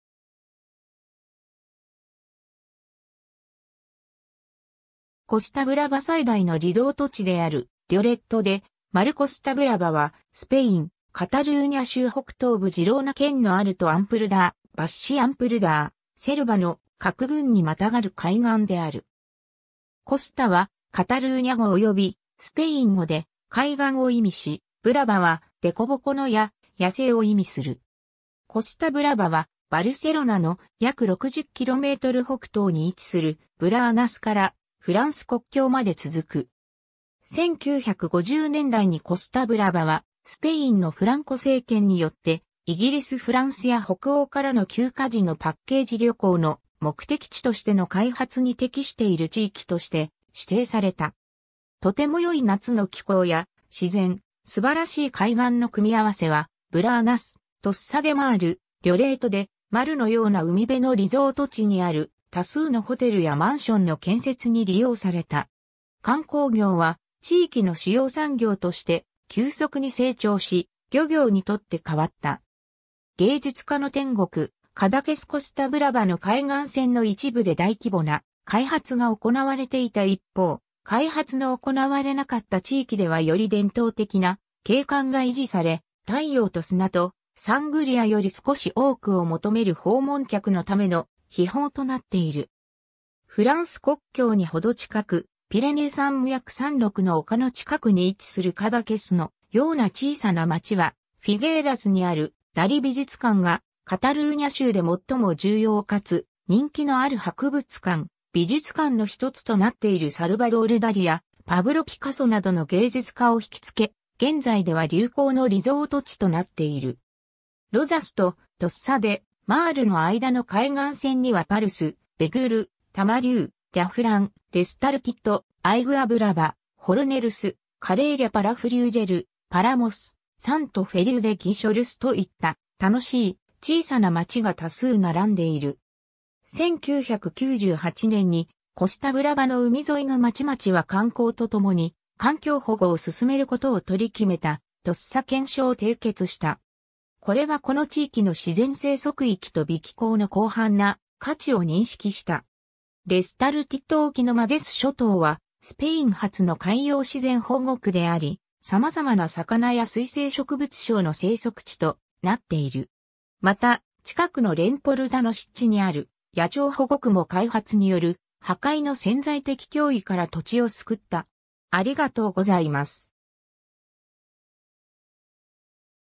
コスタ・ブラバ
コスタ・ブラバ最大のリゾート地であるリョレト・デ・マル コスタ・ブラバ（カタルーニャ語・スペイン語：Costa Brava）は、スペイン、カタルーニャ州北東部ジローナ県のアルト・アンプルダー、バッシ・アンプルダー、セルバの各郡に跨る海岸である。
コスタはカタルーニャ語及びスペイン語で「海岸」を意味し、ブラバは「でこぼこの」や「野生」を意味する。
コスタ・ブラバはバルセロナの約60km北東に位置するブラーナスからフランス国境まで続く。
 1950年代にコスタ・ブラバはスペインのフランコ政権によって、イギリス・フランスや北欧からの休暇時のパッケージ旅行の目的地としての開発に適している地域として指定された。
とても良い夏の気候や自然、素晴らしい海岸の組み合わせはブラーナス、トッサ・デ・マール、リョレート・デ・マルのような海辺のリゾート地にある多数のホテルやマンションの建設に利用された。
観光業は地域の主要産業として急速に成長し、漁業に取って代わった。
 芸術家の天国、カダケス コスタ・ブラバの海岸線の一部で大規模な開発が行われていた一方、開発の行われなかった地域ではより伝統的な景観が維持され、太陽と砂とサングリアより少し多くを求める訪問客のための「秘宝」となっている。
フランス国境に程近く、ピレネー山脈山麓の丘の近くに位置するカダケスのような小さな町は、フィゲーラスにあるダリ美術館がカタルーニャ州で最も重要かつ人気のある博物館・美術館の一つとなっているサルバドール・ダリやパブロ・ピカソなどの芸術家を惹き付け、現在では流行のリゾート地となっている。
ロザスとトッサ・デ・マールの間の海岸線にはパルス、ベグール、タマリュー、リャフラン、レスタルティット、アイグア・ブラバ、フォルネルス、カレーリャ・パラフリュージェル、パラモス、サント・フェリウ・デ・ギショルスと言った楽しい小さな町が多数並んでいる。
 1998年にコスタ・ブラバの海沿いの町々は観光と共に環境保護を進める事を取り決めたトッサ憲章 (Carta de Tossa) を締結した。
これはこの地域の自然生息域と微気候の広範な価値を認識した 。
レスタルティット沖のマデス諸島はスペイン初の海洋自然保護区であり、様々な魚や水生植物相の生息地となっている。
また、近くのレンポルダの湿地にある野鳥保護区も開発による破壊の潜在的脅威から土地を救った。